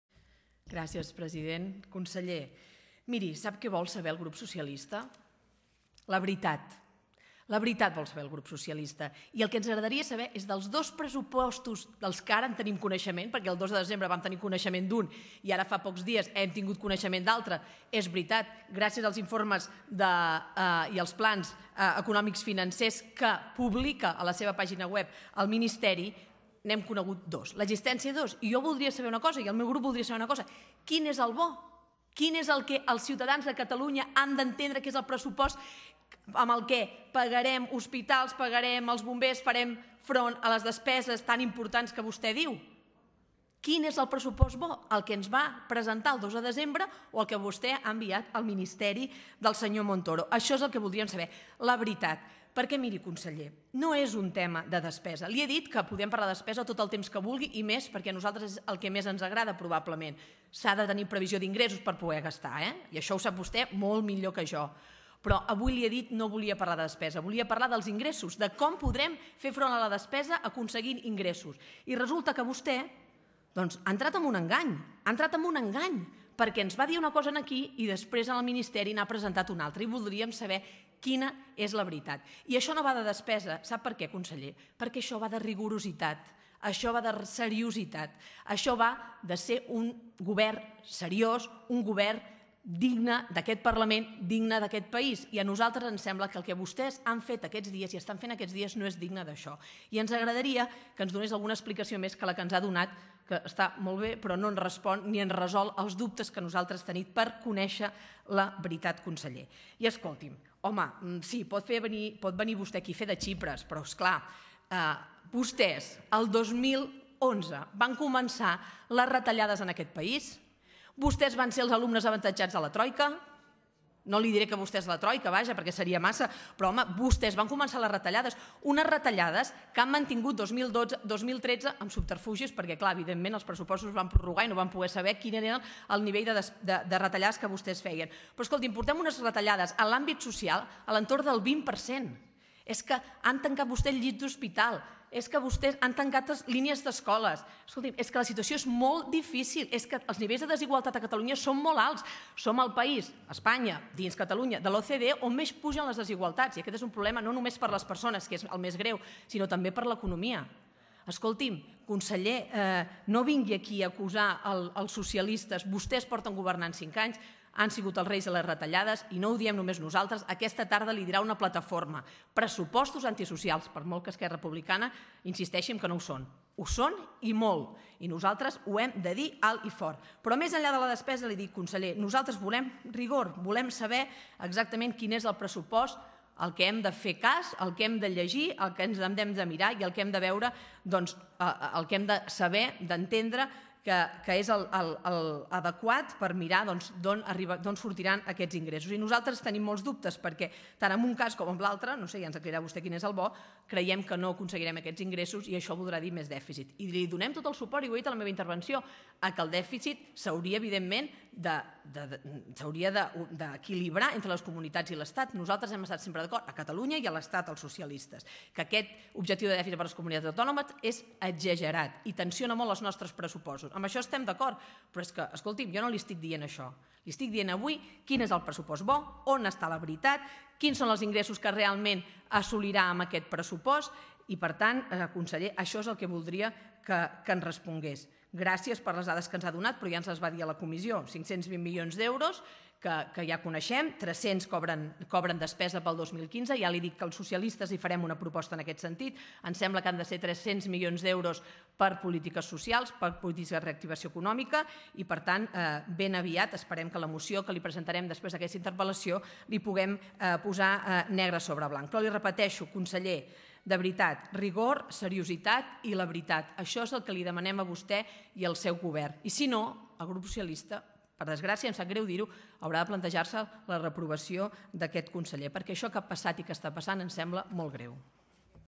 On és la veritat, als pressupostos que CiU presenta al Parlament o als que envia a Montoro?
La diputada portaveu d’Economia del Grup Parlamentari Socialista, Alícia Romero, ha interpel·lat aquest dimecres al Conseller Mas-Colell per què aclareixi d’una vegada “a qui ha enganyat?”: si al Parlament de Catalunya quan va presentar la seva proposta de pressupost el mes desembre passa, o al Ministeri d’Hisenda ara que ha presentat a Madrid el Pla Econòmic Financer per a Catalunya.

Alícia Romero ha posat en evidència en la seva intervenció que “mentre que aquí se’ns diu que s’ingressaran 2.200 milions d'euros amb els deutes provinent de l’Estat, al Pla Econòmic Financer que s’envia a Madrid, en canvi, aquests milions es converteixen en previsió d’ingressos per venta de patrimoni”.

“A quin pressupost hem de fer cas? –ha qüestionat Romero al conseller– On és la veritat? Quins són els ingressos que realment assolirà aquest pressupost?”.

Alícia Romero ha denunciat en la seva interpel·lació que, en tot cas, sigui quina sigui la previsió d’ingressos verdadera, els pressupostos de la Generalitat seguiran essent “irresponsables, antisocials i no resoldran els problemes de la gent” perquè consoliden les retallades que van començar amb els Govern de CiU el 2011.

“Vostès van ser alumnes avantatjats de la Troica, acumulem retallades en l’àmbit social entorn del 20% del pressupost; els nivells de desigualtat a Catalunya són molt alts i creixen; i tot això no ho diem només nosaltres, aquesta tarda li ho dirà la Plataforma per a una Catalunya Social contra els pressupostos antisocials”.

Alícia Romero ha qualificat com de “molt greu” la situació generada per aquestes previsions d’ingressos que aquí són unes i que quan s’envien a Madrid són unes altres. “És molt greu el que ha passat i el que està passant. No és rigorós i no és digne del nostre país. I si no tenim respostes clares, el grup socialista ens haurem de plantejar, molt a desgrat nostre, la reprovació del Conseller”.